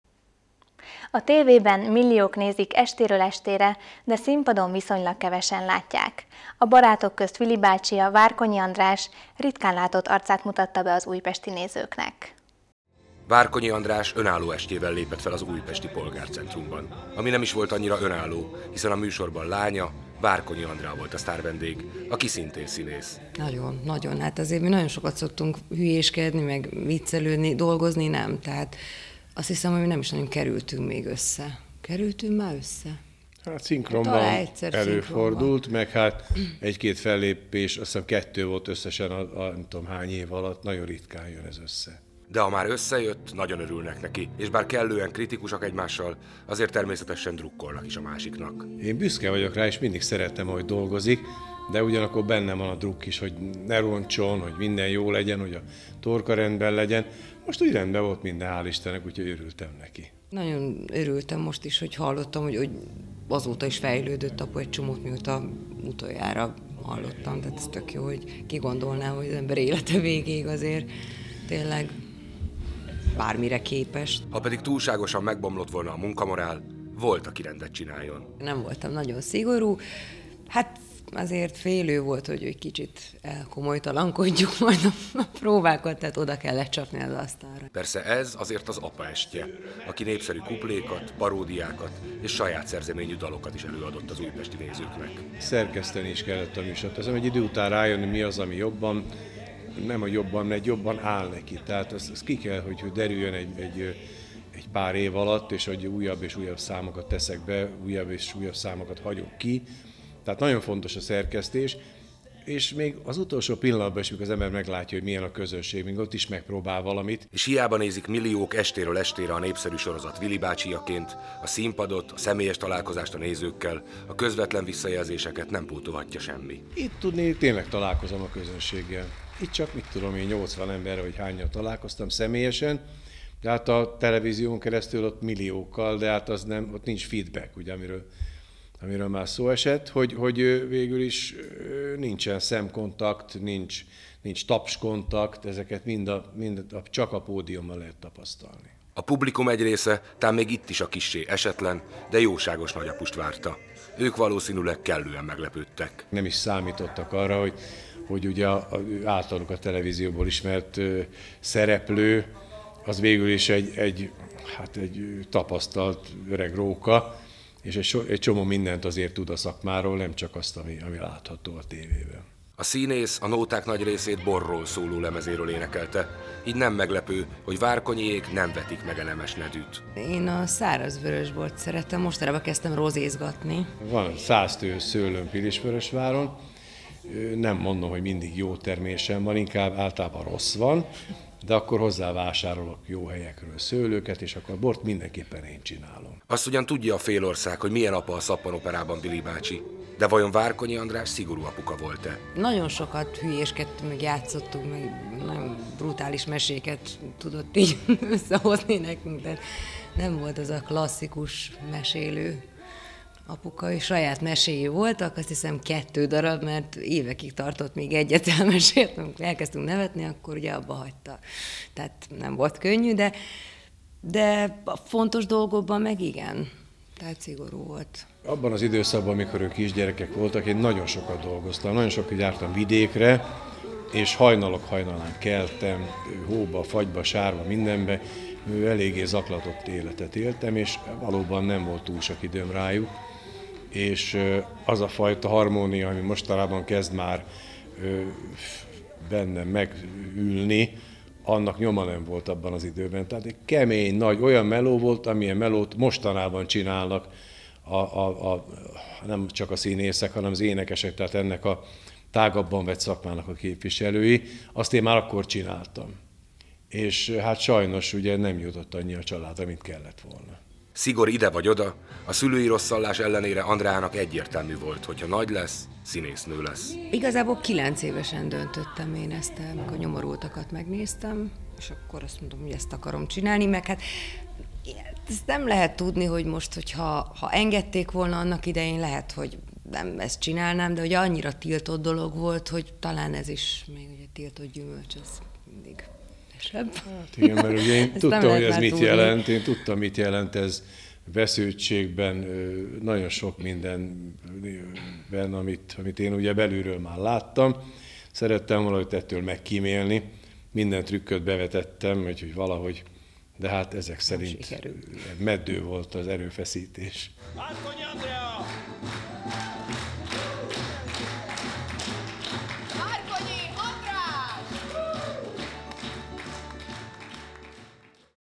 Várkonyi András és lánya lépett fel Újpesten
"Részlet az Újpesti Közéleti Televízió 2011. november 25-i adásából"

A tv-ben milliók nézik estérő estére, de színpadon viszonylag kevesen látják. A Barátok közt Vili bácsija, Várkonyi András ritkán látott arcát mutatta be az újpesti nézőknek.